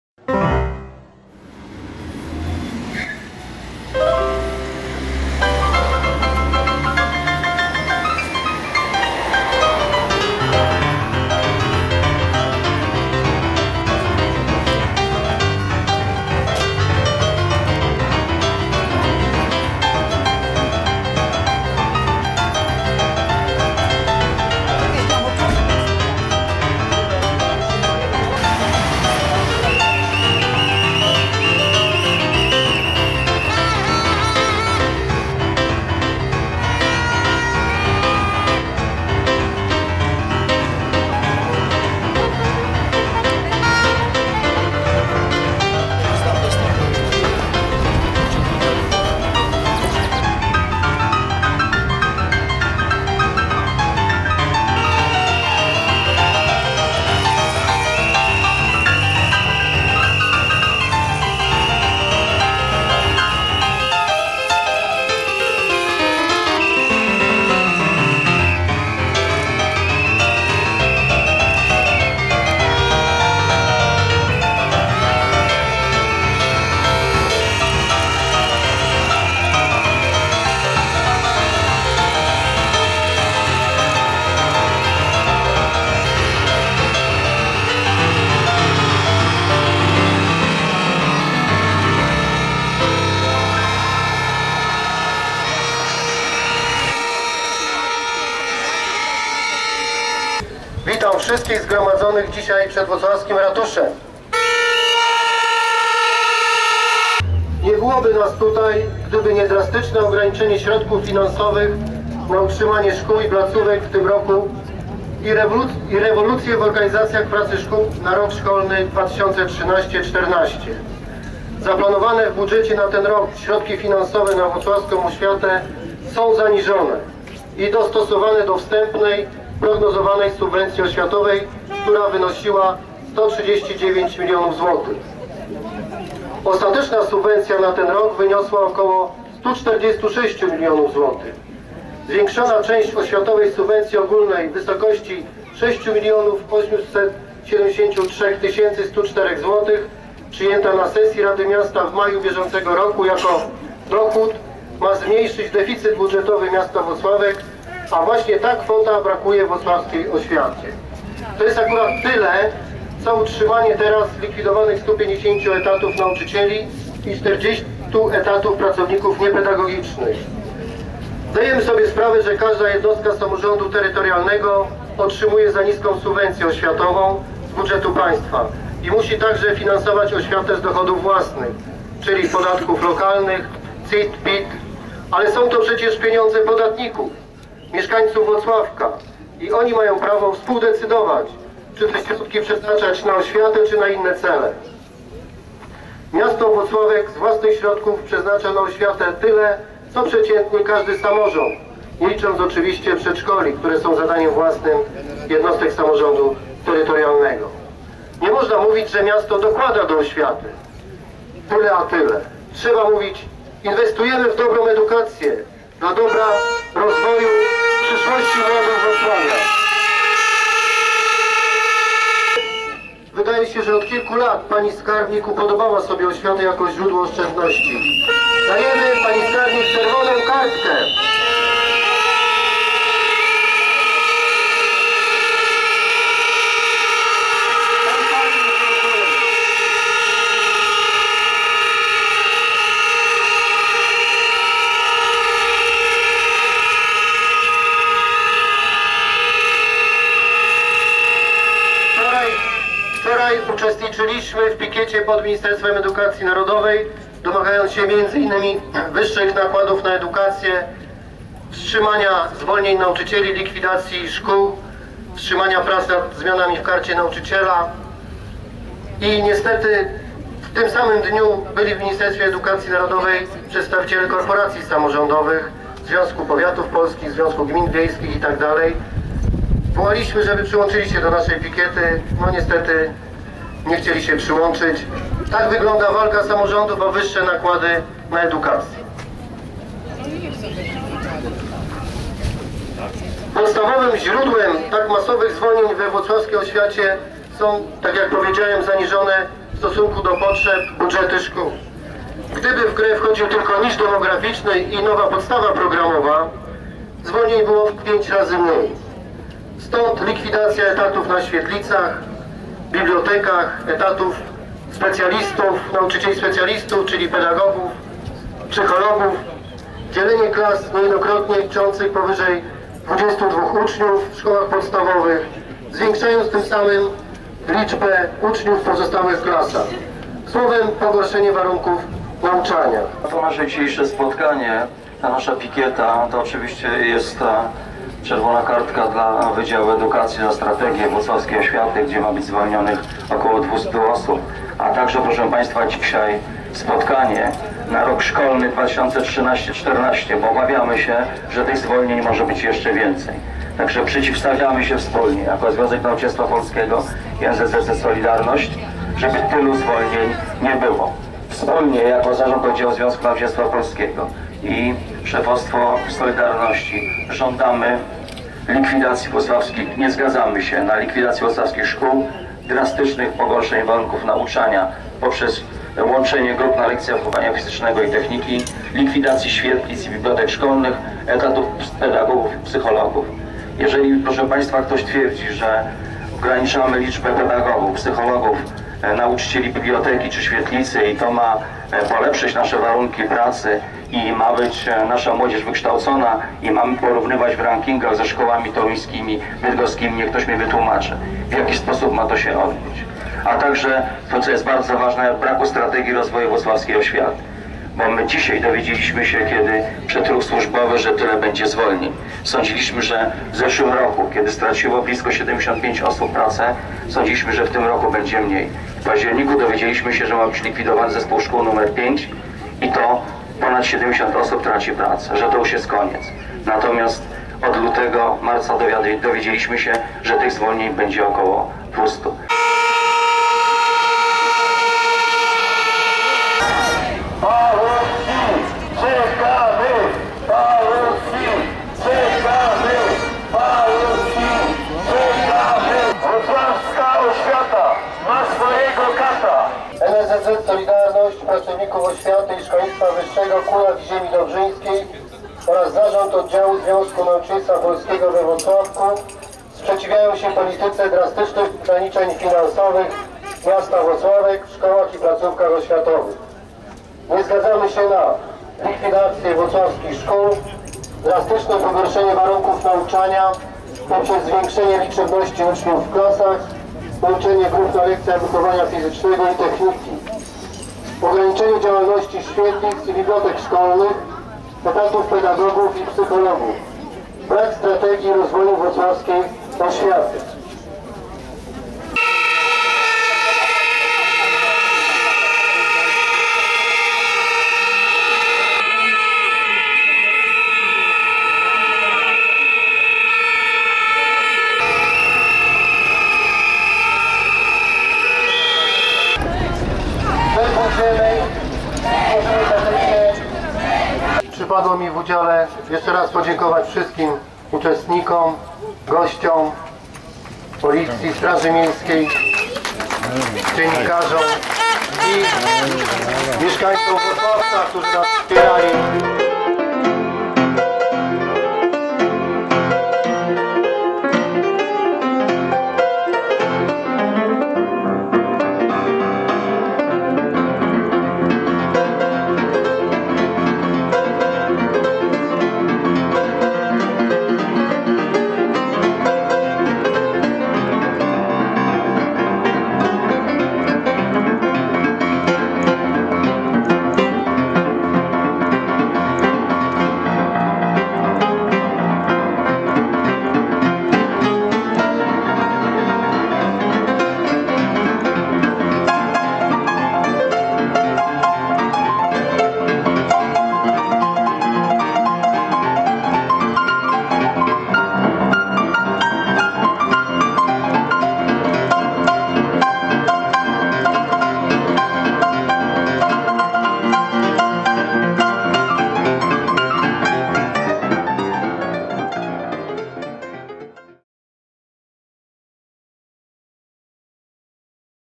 Pikieta pracowników włocławskiej oświaty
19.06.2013, przed Urzędem Miasta Włocławek miała miejsce pikieta związków zawodowych oświaty. Ten film to skrócony zapis tego co się tam działo...